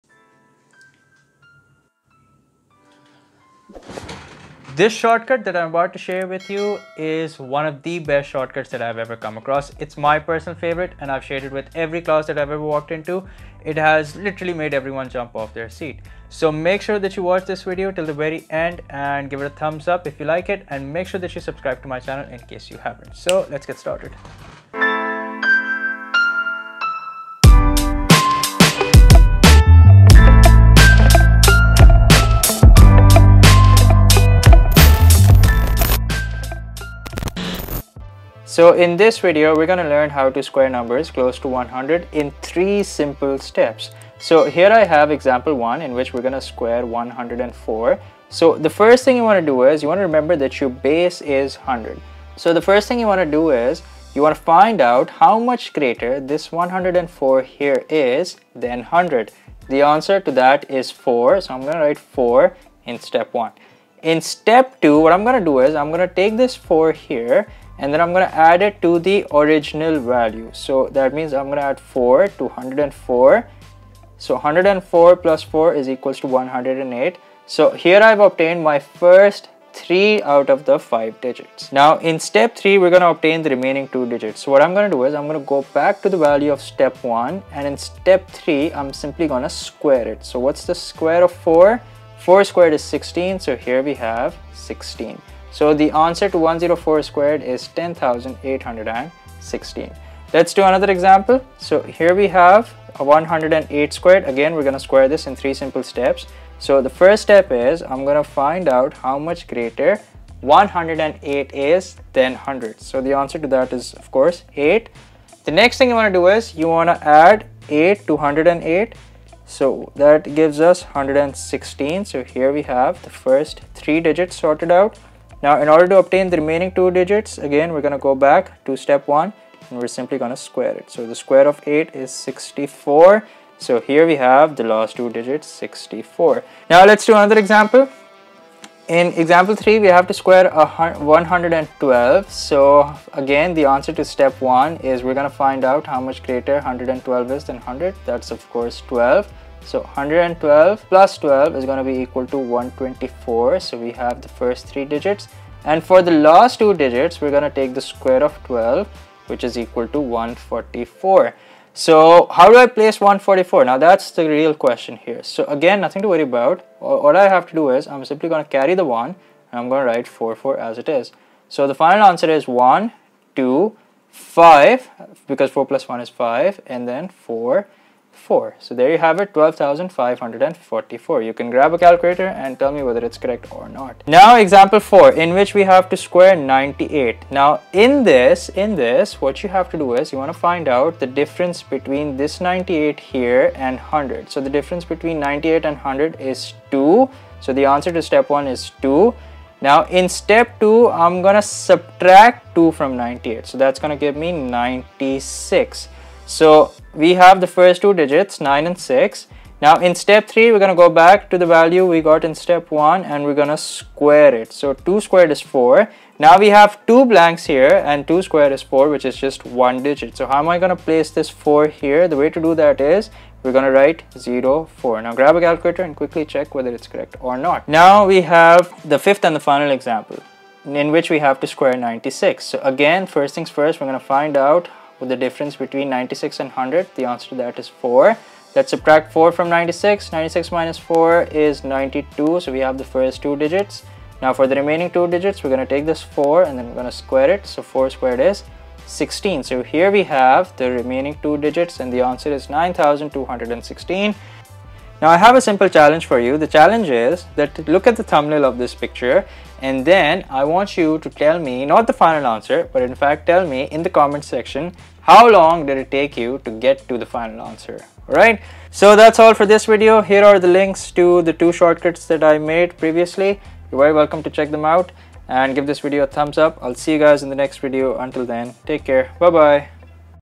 How to Square Numbers fast without a Calculator in 3 seconds - Part 3
In this video, we will learn how to find the square of numbers close to hundred.
Socials
Where do I teach?